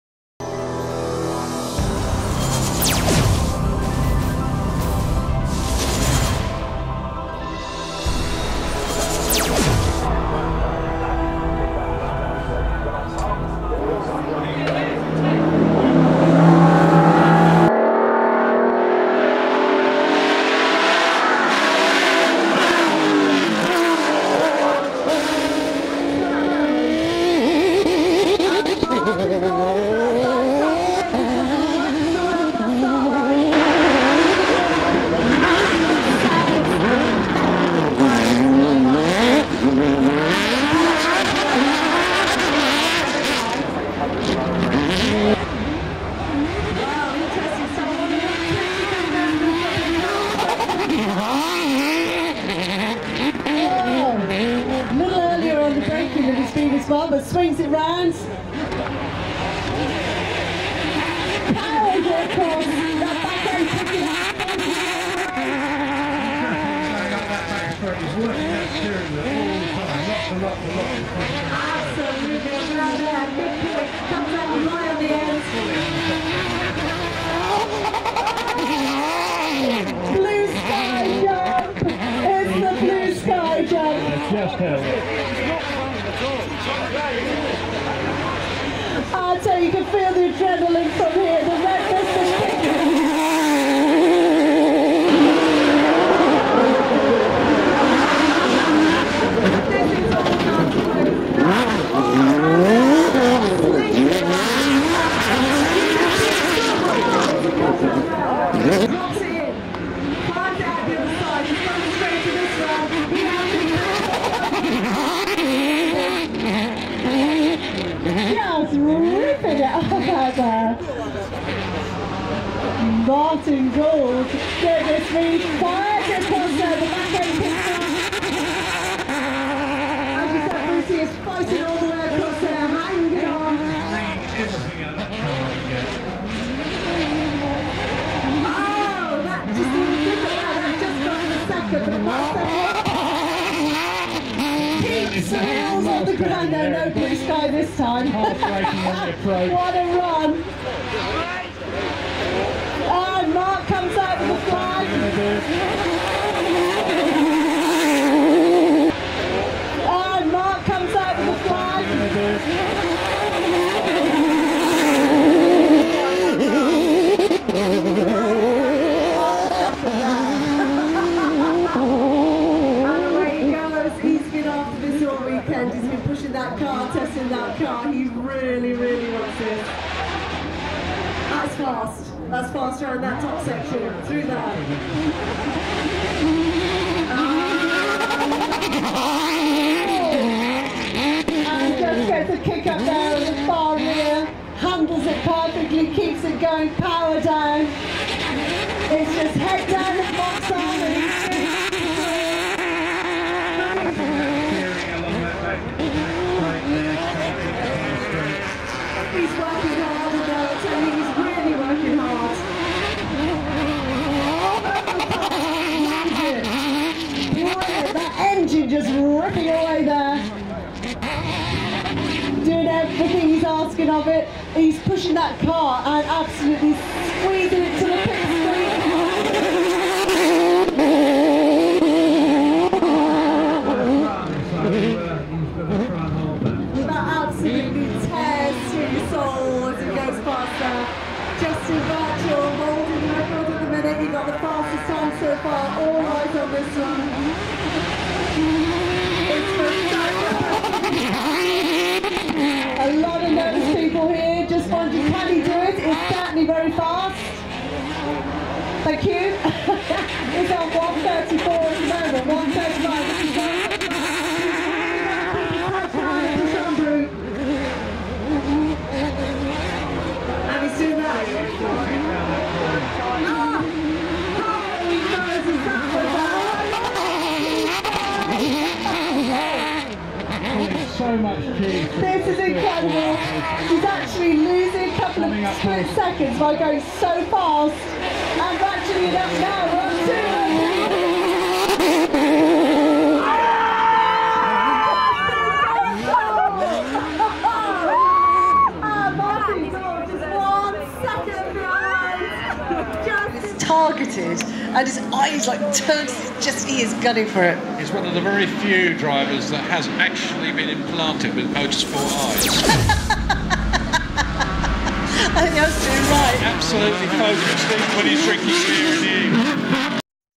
Goodwood Festival Of Speed 2019 - Britpart Arena Martin Gould
A short video showing action from Martin Gould in the RM Rivet BMW M3 at the Goodwood Festival of Speed during July 2019.
Une courte vidéo montrant l'action de Martin Gould dans la BMW Rivet BMW M3 du Goodwood Festival of Speed en juillet 2019.
Camera John Hilton